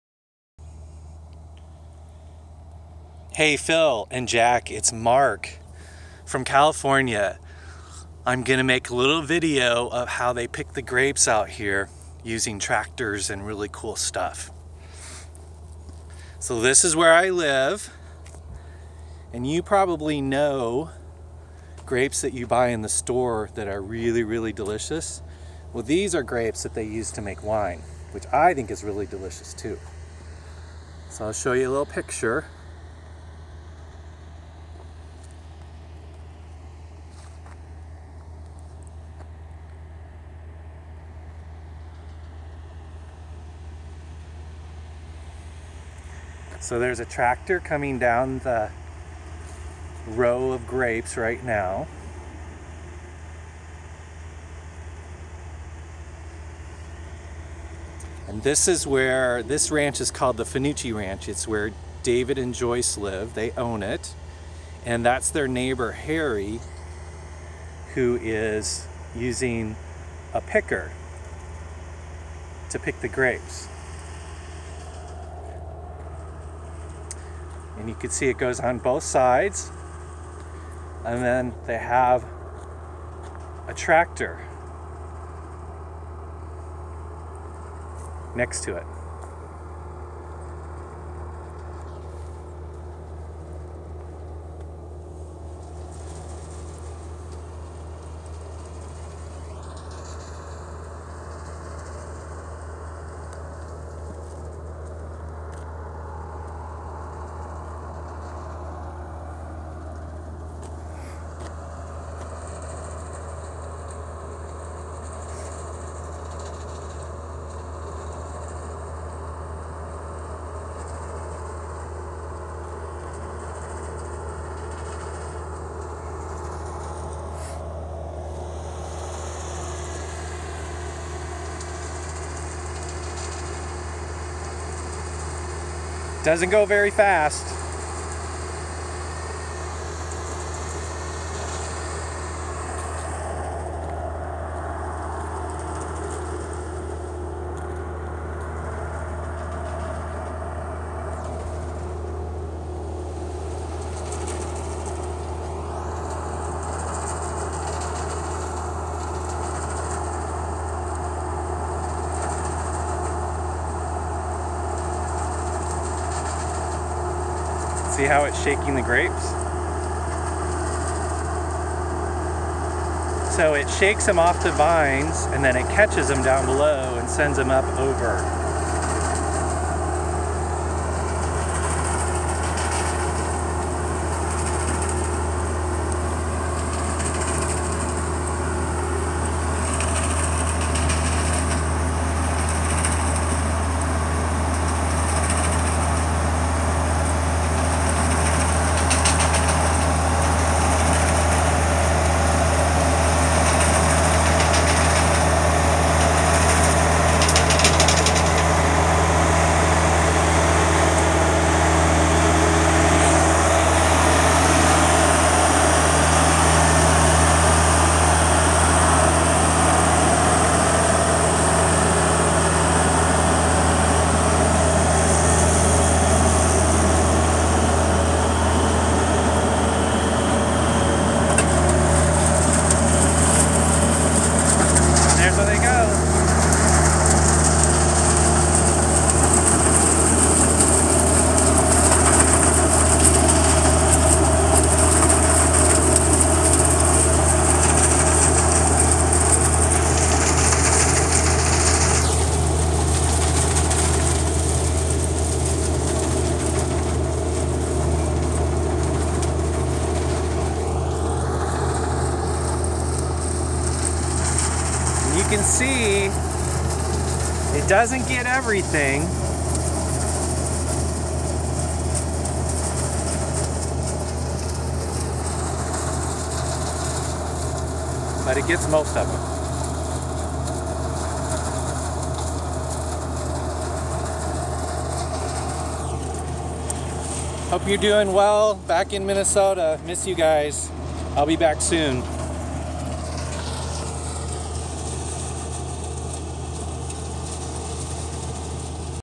Picking Grapes
Harvest time on the Fanucchi Ranch in Alexander Valley.